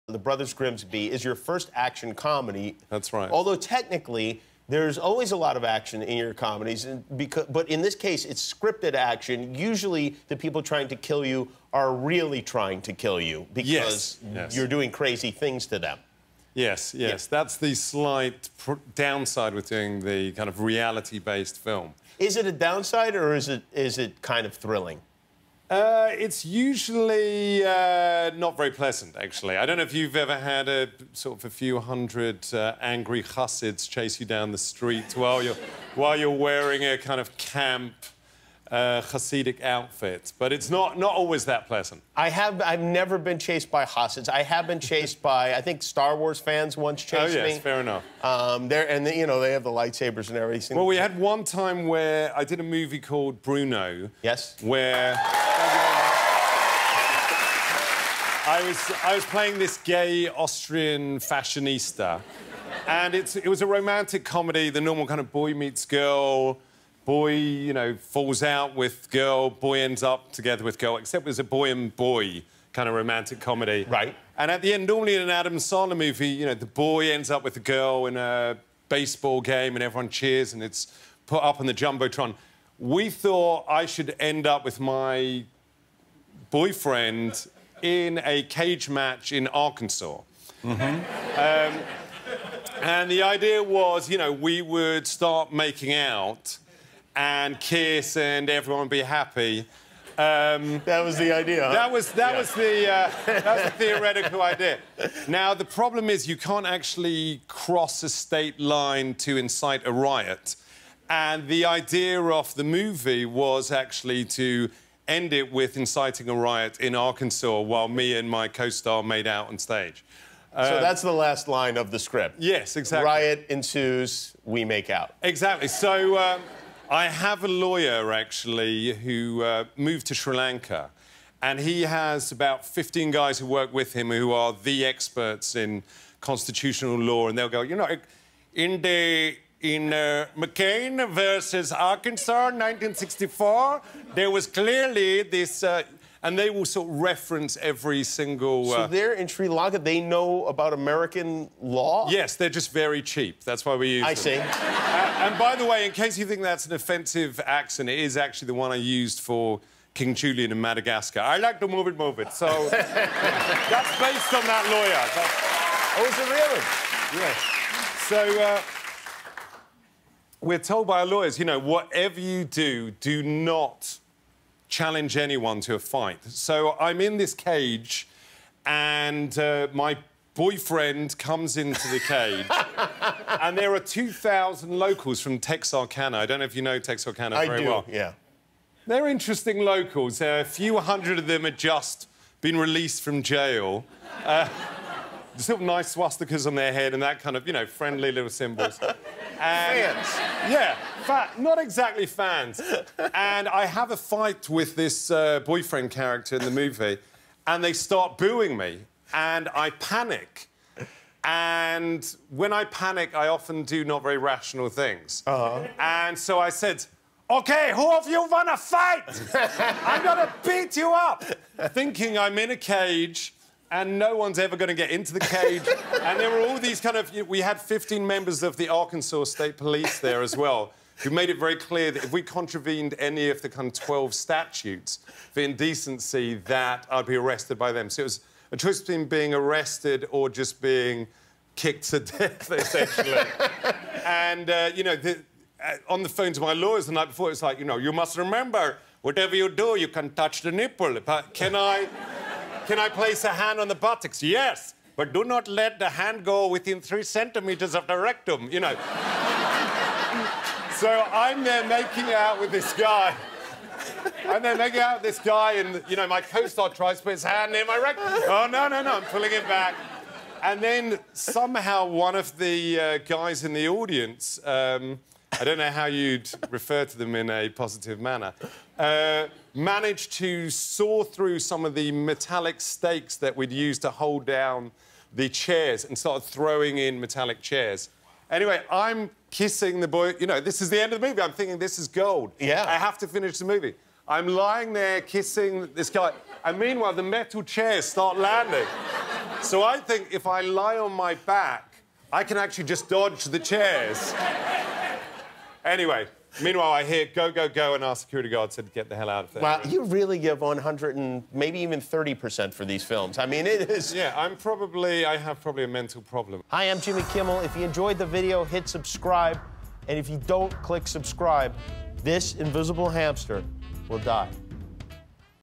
Sacha Baron Cohen Incited a Riot While Making “Bruno"
Sacha recalls the riot that erupted while he was shooting a scene for the movie “Bruno.”